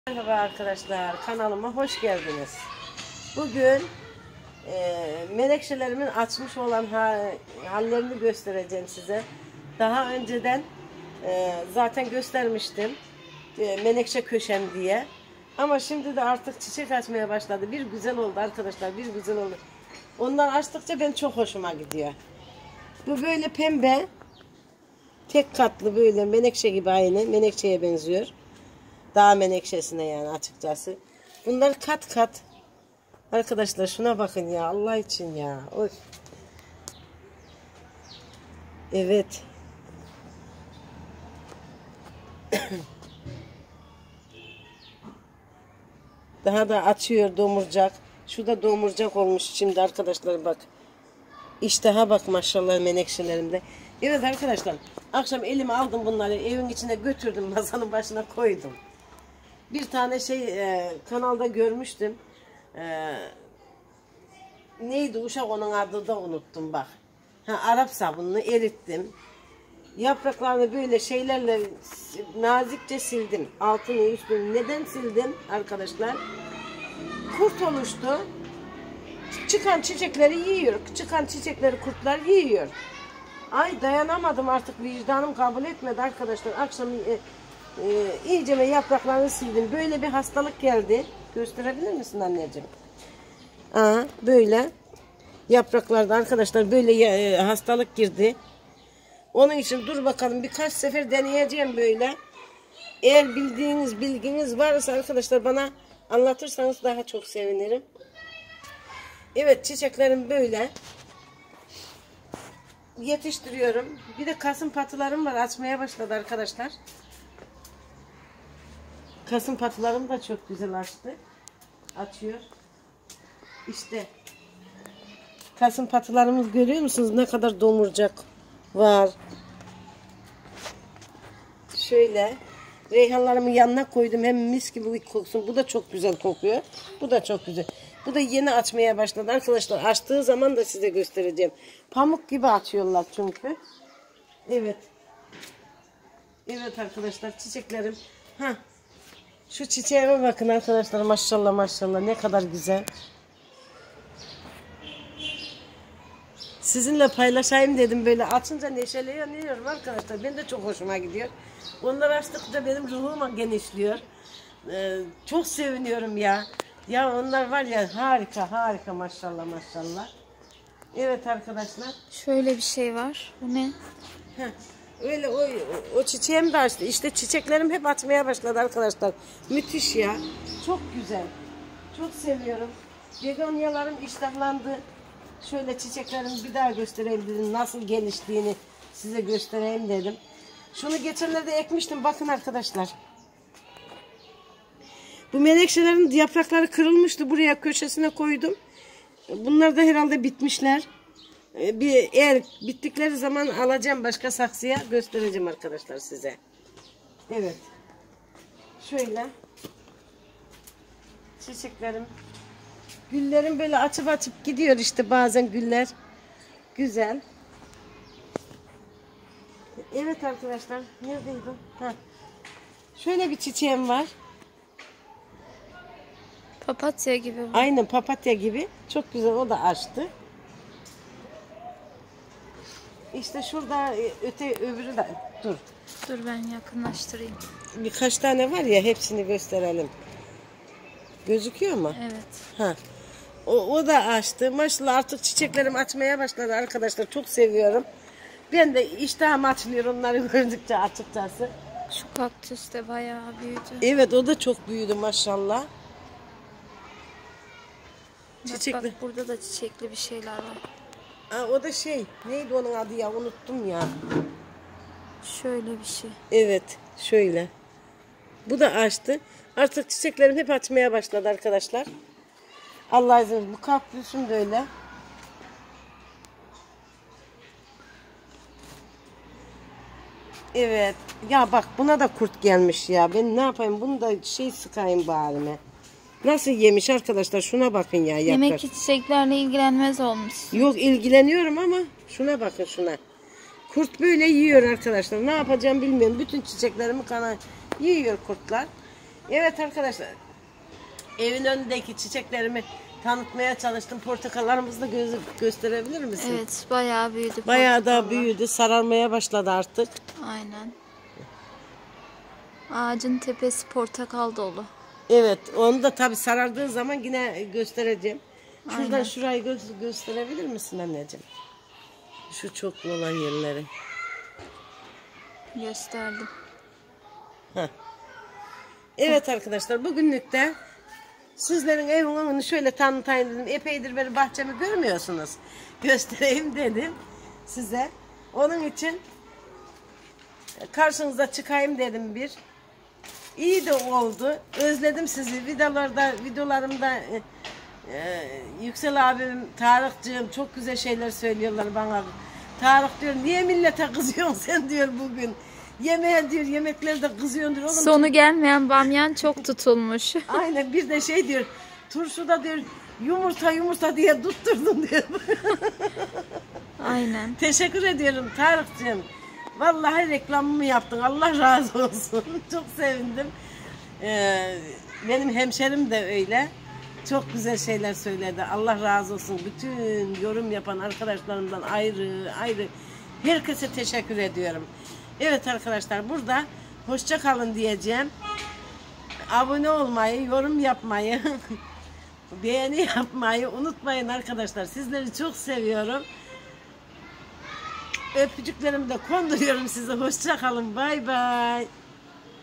MENEKŞELERİM ÇIÇEK AÇTI / DOSTLARA SELAM
Merhaba arkadaşlar,
menekşelerim çiçek açtı. Sizlerle hem ufak bir sohbet gerçekleştirdim, hemde çiçek açan menekşelerimi görmenizi istedim. Bahçemi çok seviyorum ve sevdiğim yerden sevdiğim dostlarıma selam yolladım. Gelin bakın menekşelerim nede güzel çiçek açtılar.

Menekşe Çiçeği
Menekşe Nasıl Çiçek Açar
Menekşe Çiçeği Nasıl Olur